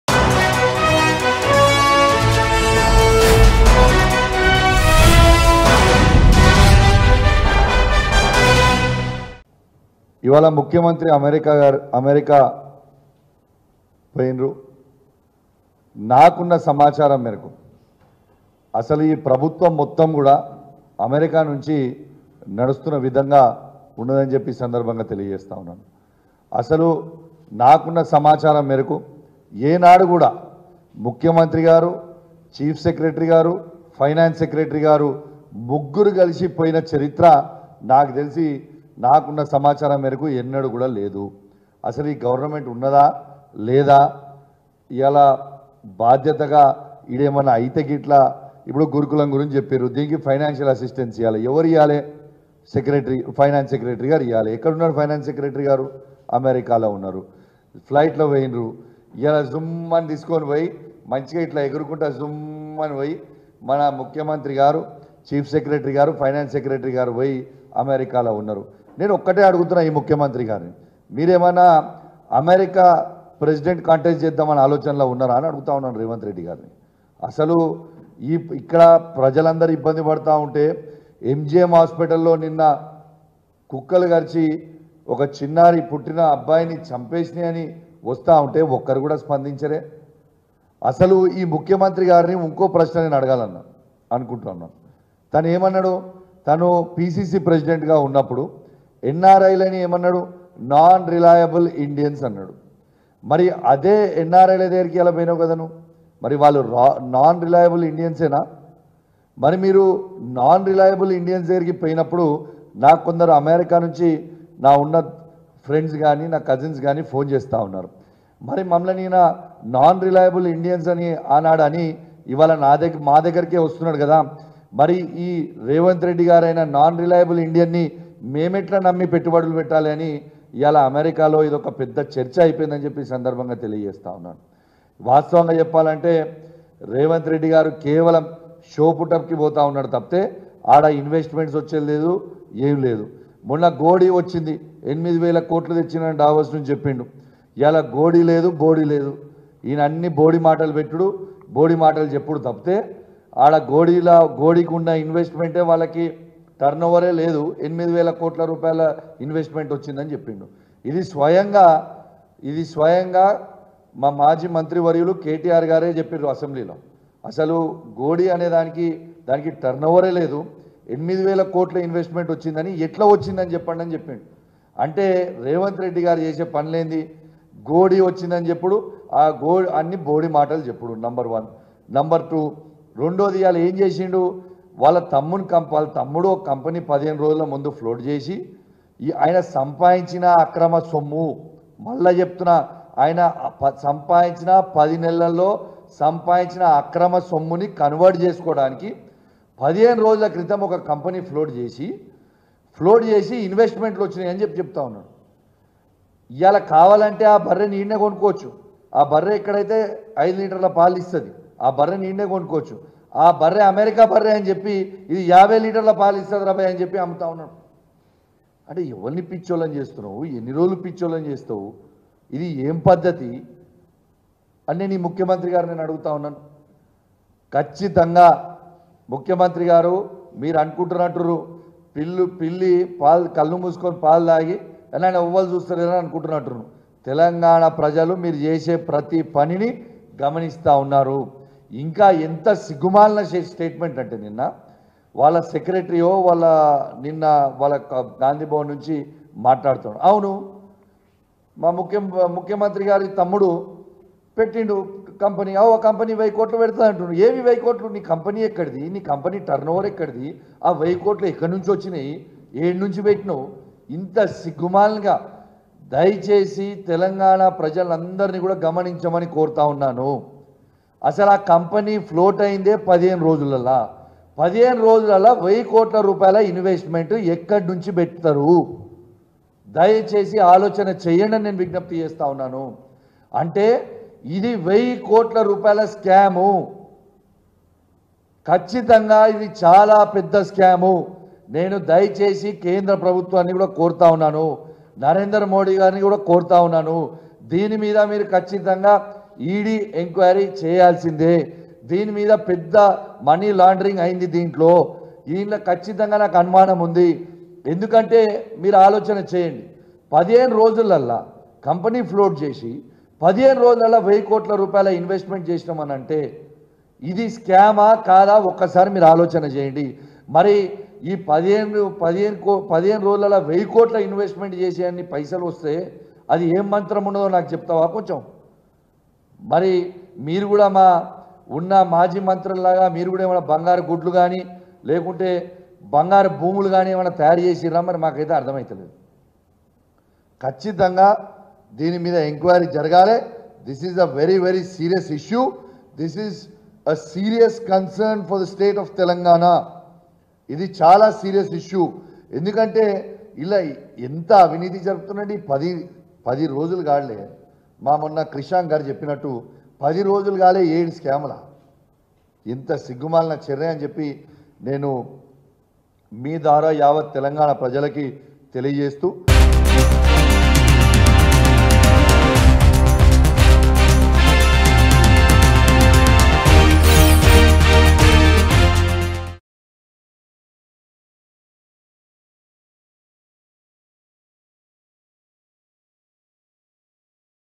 Padi Kaushik Reddy Slams CM Revanth Reddy USA Tour | Telangana Politics | TG News | Mango News
Padi Kaushik Reddy Slams CM Revanth Reddy USA Tour. Padi Kaushik Reddy criticized CM Revanth Reddy's America tour, questioning its significance for local issues.